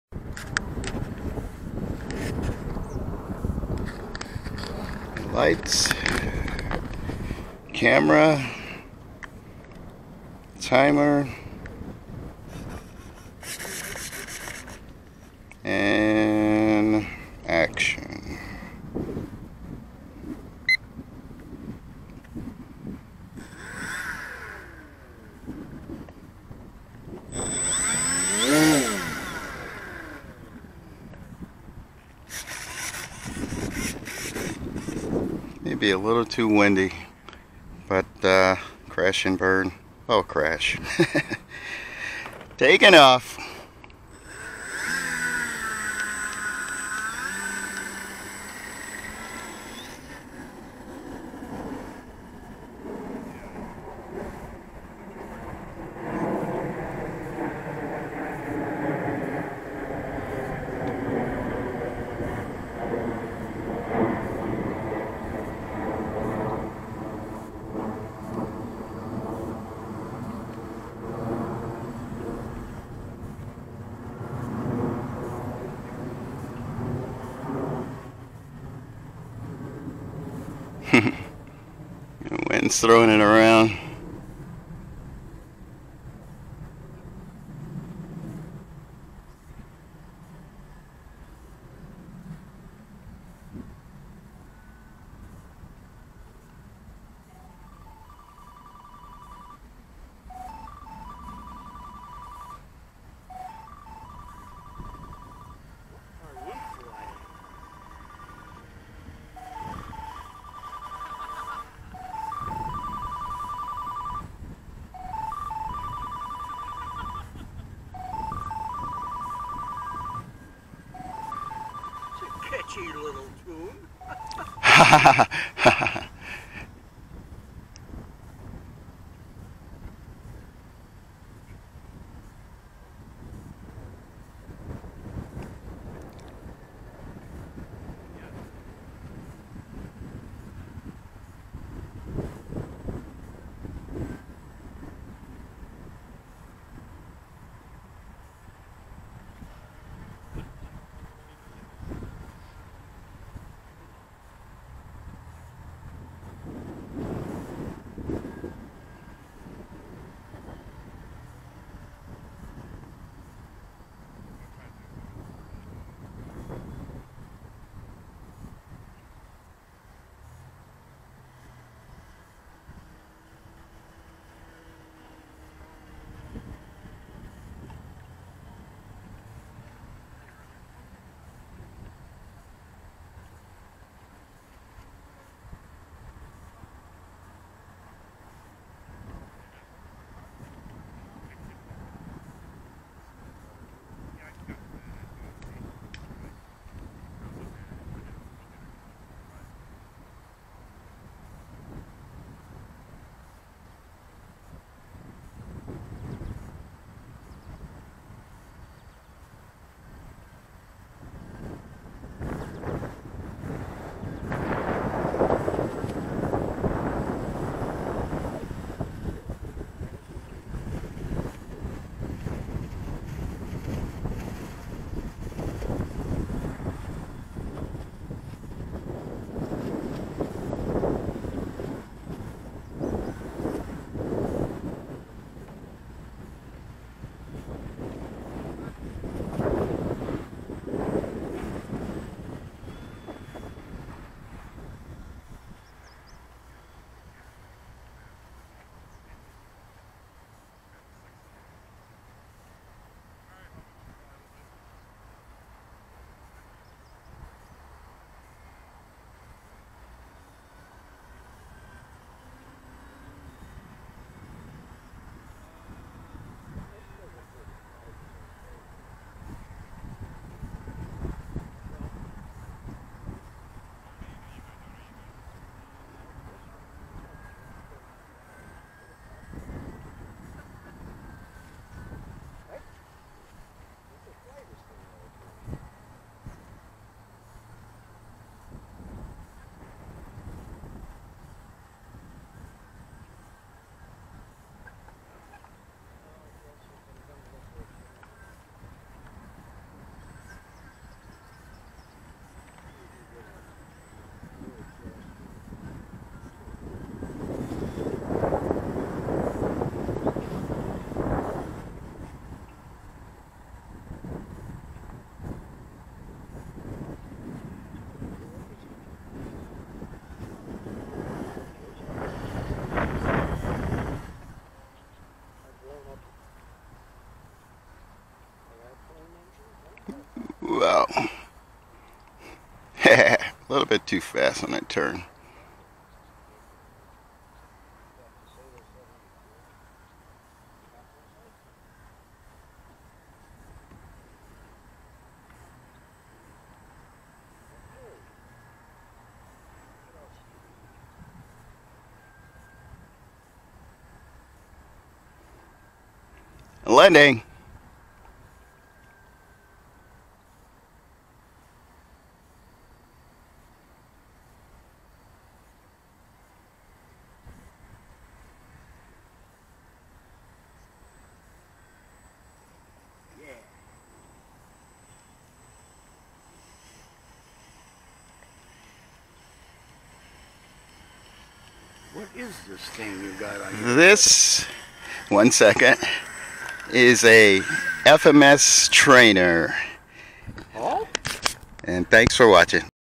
FMS Super EZ (((wind gust)))
I sometimes like flying in wind, it helps improve your skills; not saying I'm good yet lol, I do ok.